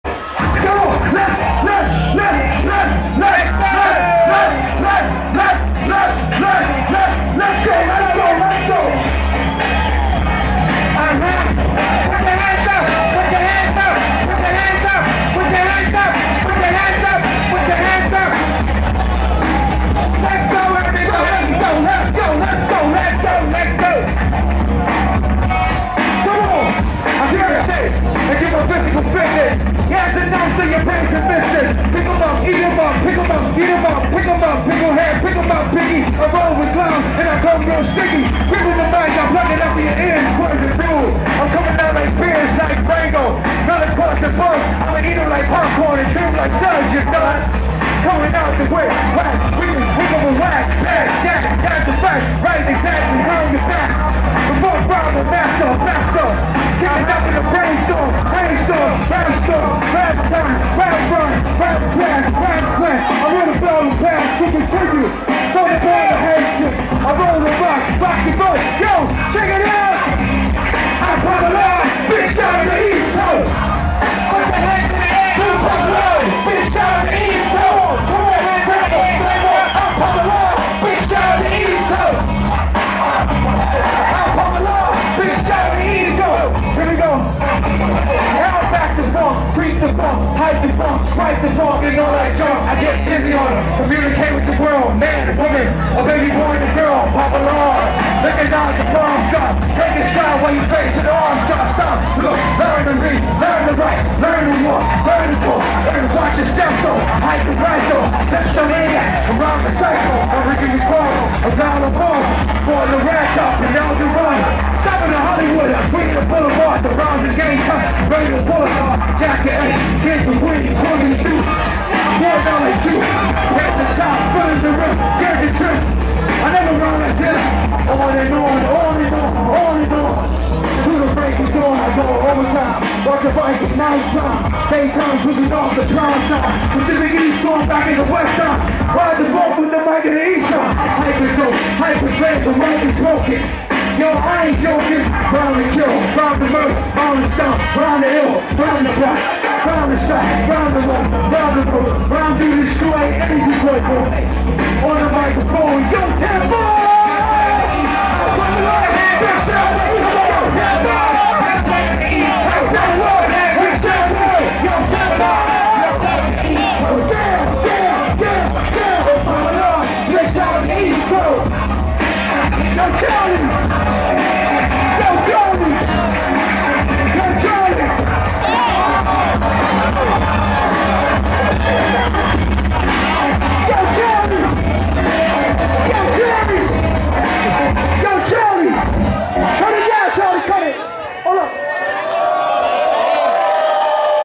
Kool Keith vs 50 drunk and high skaters.
The LEGEND survived unharmed. Great show.